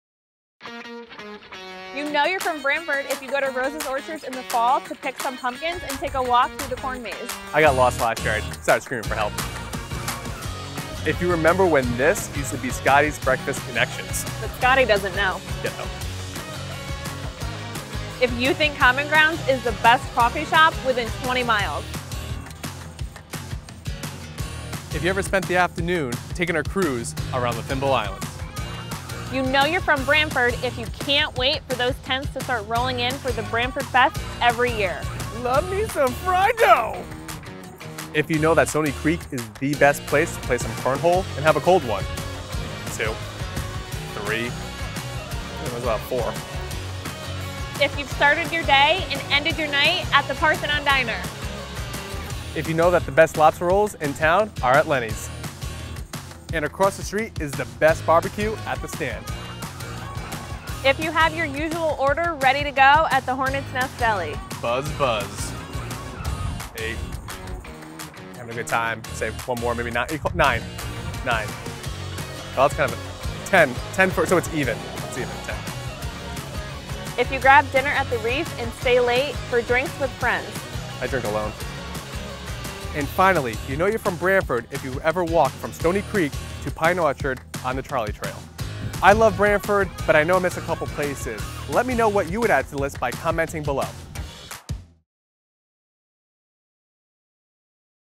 You Know You're From Branford, CT if...
You know you’re from Branford…

...if you go to Rose Orchards in the fall to pick some pumpkins and take a walk through the corn maze

...if you remember when Breakfast Connections of Branford used to be Scotty's Breakfast Connections

...if you think Common Grounds is the best coffee shop within 20 miles

...if you ever spent an afternoon taking a cruise around the Thimble Islands

You know you’re from Branford if you can’t wait for those tents to start rolling in for The Branford Festival every year

...if you know that Stony Creek Beer is the best place to play some cornhole and have a cold one...or two...or three...

...if you’ve started your day and ended your night at the Parthenon Diner Restaurant

...if you know that the best lobster rolls in town are at Lenny's Indian Head Inn

...and across the street is the best BBQ at The Stand

...if you have your usual order ready to go at the Hornet's Nest Breakfast, Lunch and Catering

...if you grabbed dinner at the Donovan's Reef Restaurant & Catering and stayed late for drinks with friends

Finally, you know you’re from Branford if you’ve ever walked from Stony Creek to Pine Orchard on the Branford Trolley Trail

We love Branford, but we know we’ve missed some places. Let us know what you would add below!